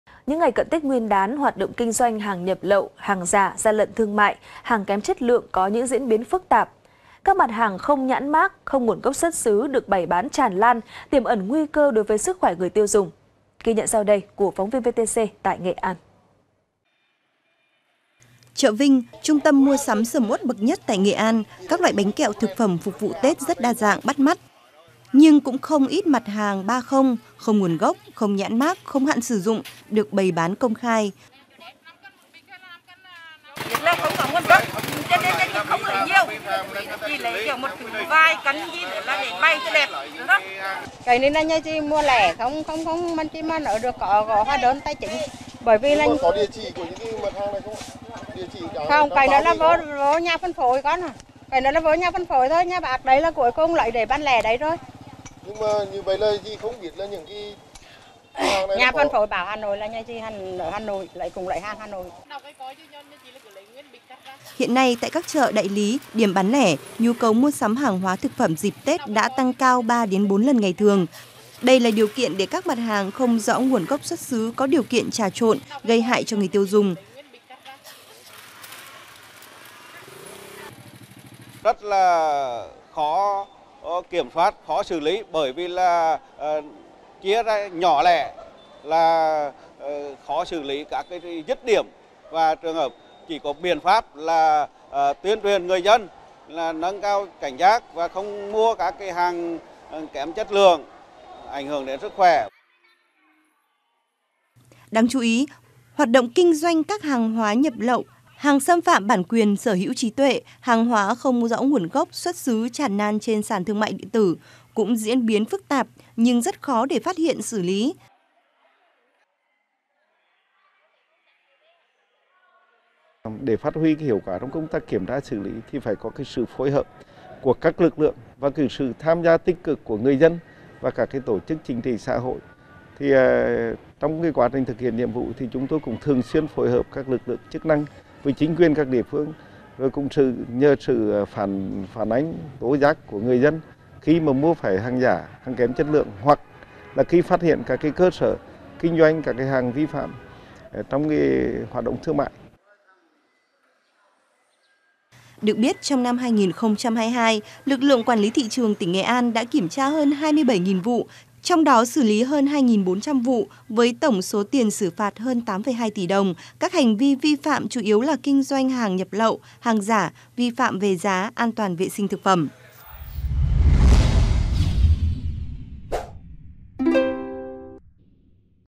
Hàng hóa không rõ nguồn gốc trà trộn trên Thị Trường| VTC14
VTC14 |Những ngày cận tết nguyên đán, hoạt động kinh doanh hàng nhập lậu, hàng giả, gian lận thương mại, hàng kém chất lượng có những diễn biến phức tạp. Các mặt hàng không nhãn mác, không nguồn gốc xuất xứ được bày bán tràn lan, tiềm ẩn nguy cơ đối với sức khỏe người tiêu dùng.